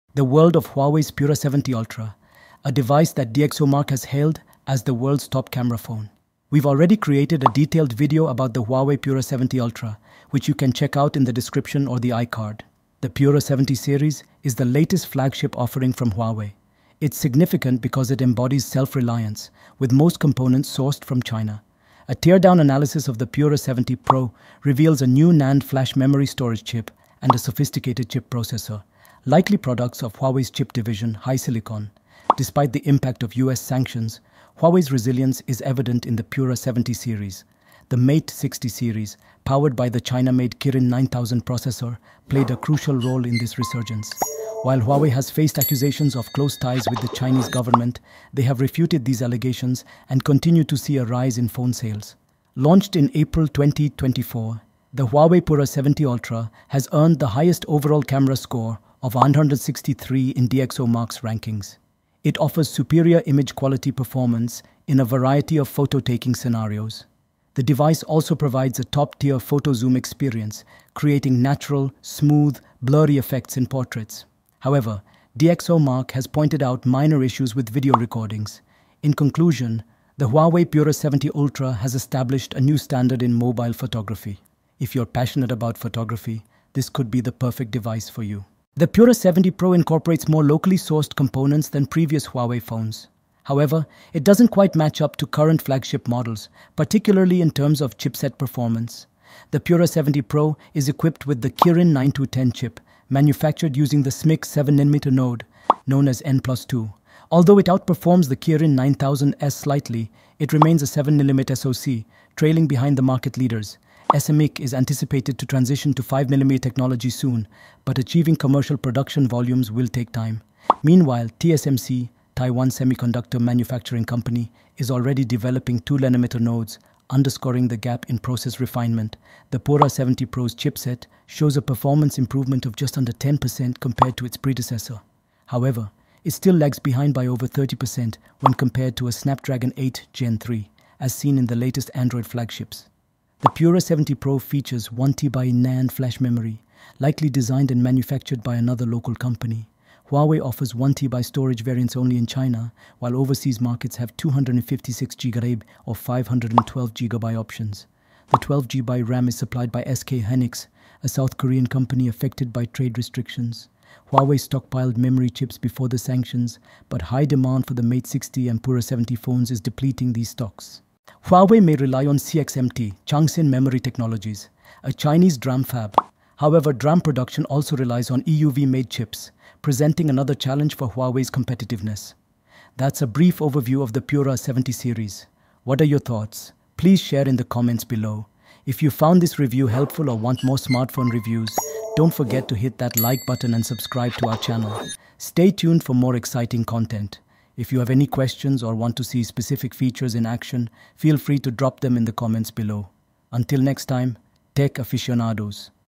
Huawei Pura 70 Pro: Showcasing China’s Tech Prowess
The world of Huawei's Pura 70 chipset and RAM update with this in-depth YouTube video. Discover how these upgrades can elevate your smartphone experience to the next level. Get a closer look at the features and improvements that come with this latest release.

Cheers
Sidzai Tech :)